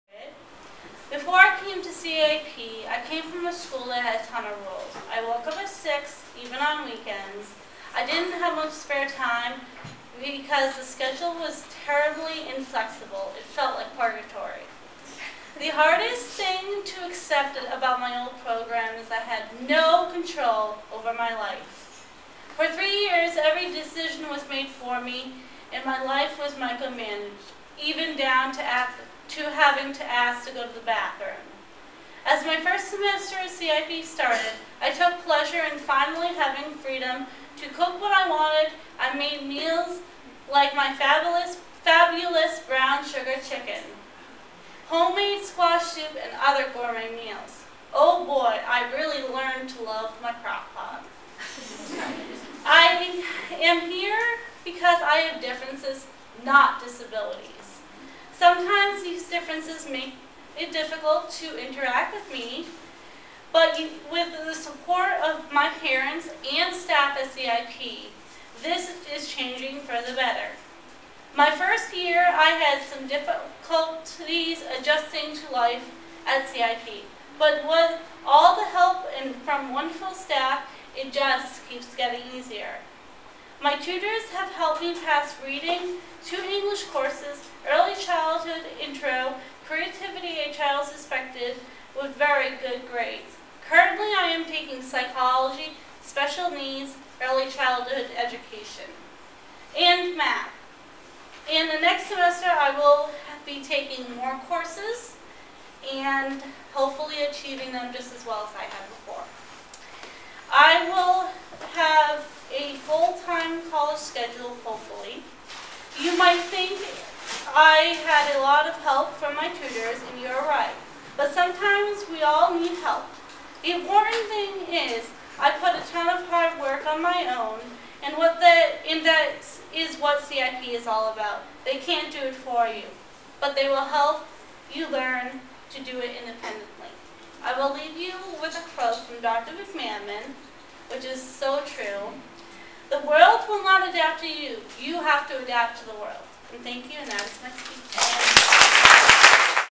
CIP Berkshire: Erin's Parents Weekend Speech 2012
Erin gives a speech during the 2012 parent weekend at CIP Berkshire.